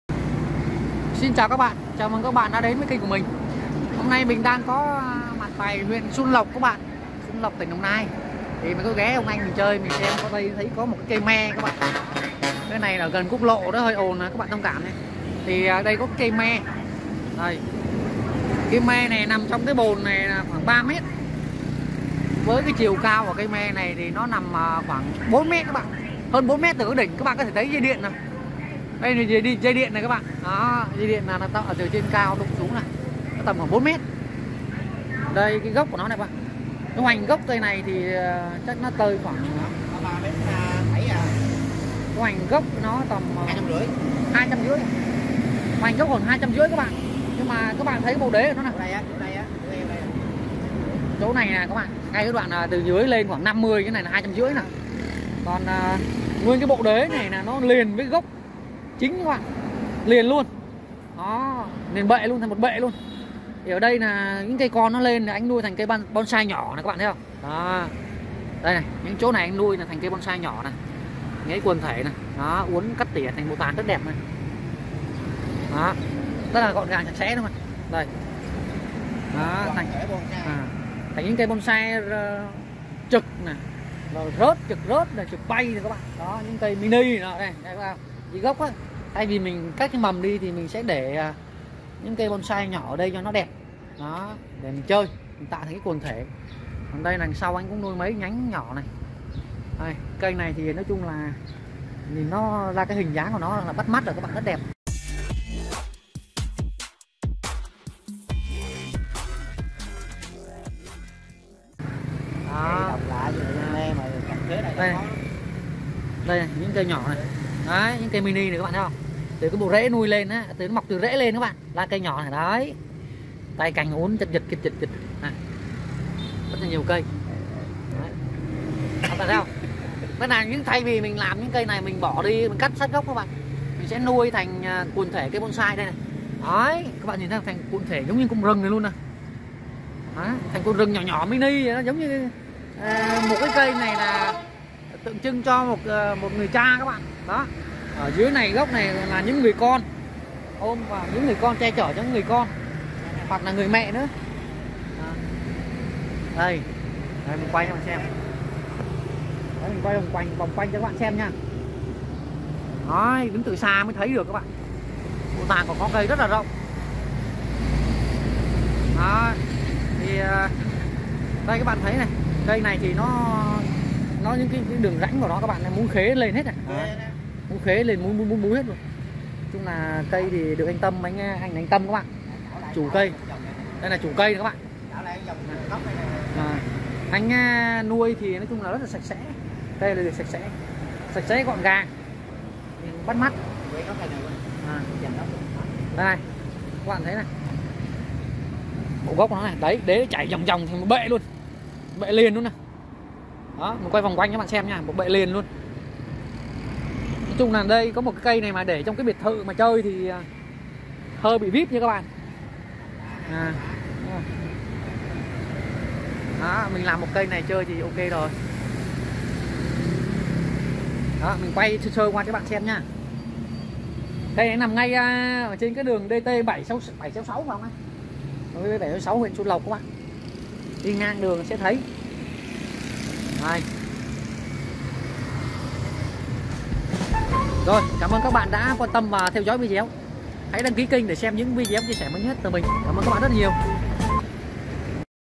Cây me bonsai siêu khủng
Hôm nay mình có ghé xuân lộc đồng nai xem cây me bonsai siêu khủng.mình có quay lại cho các bạn xem.